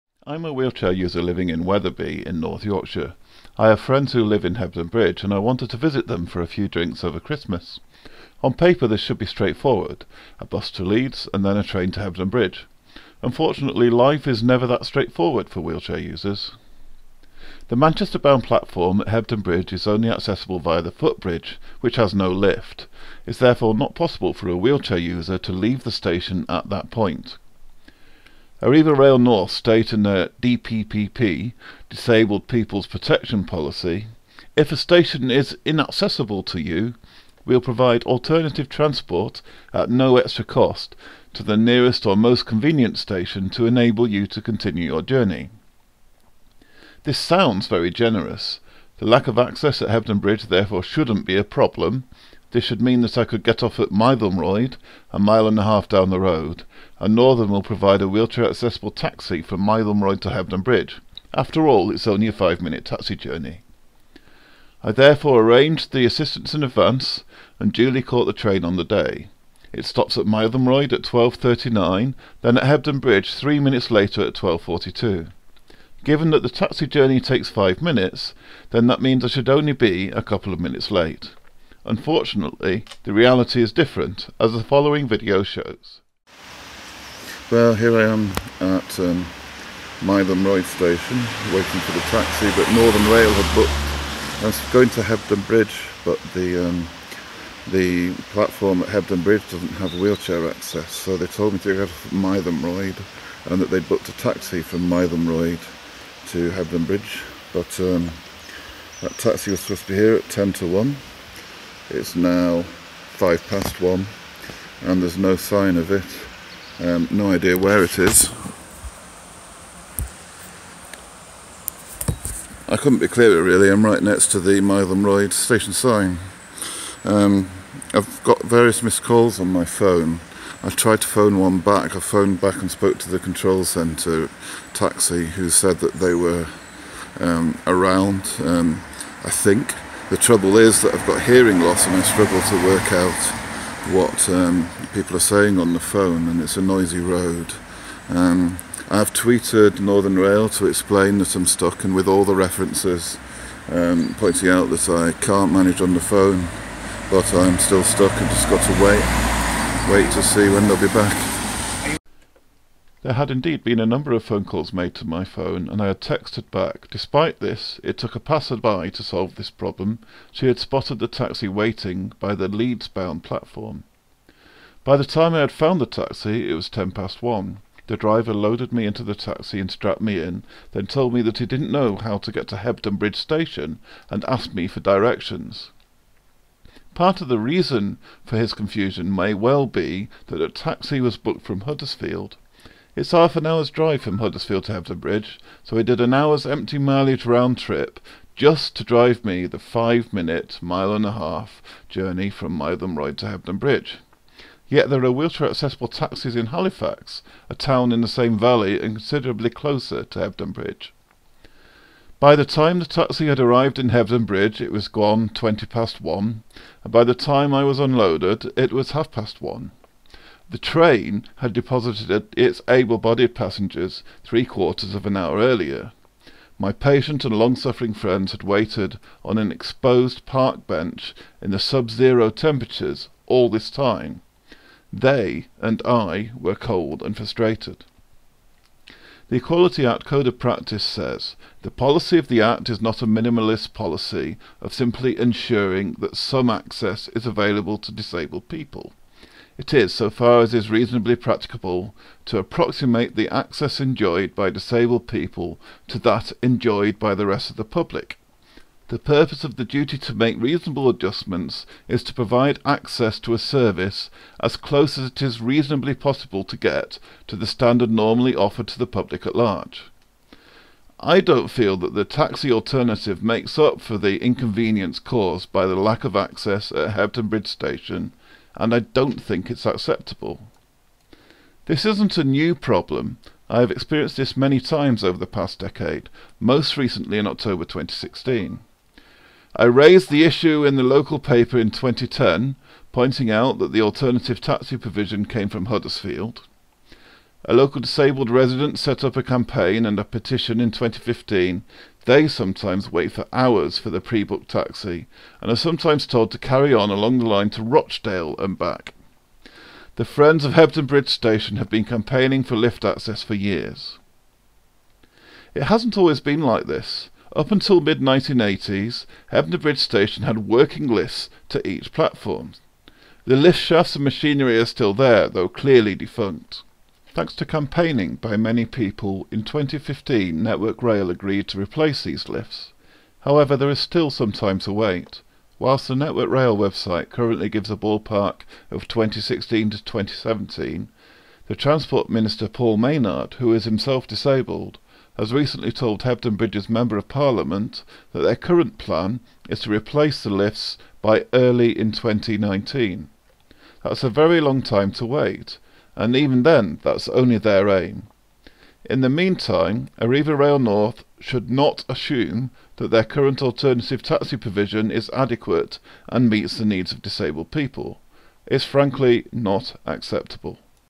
Hebden Bridge station: wheelchair access and alternative taxis
The Manchester-bound platform at Hebden Bridge station isn't wheelchair accessible. The lift machinery has been out since the mid 1980s and it won't be replaced until early 2019. Arriva Rail North provide an alternative accessible taxi from Mytholmroyd, but this isn't satisfactory, as I show.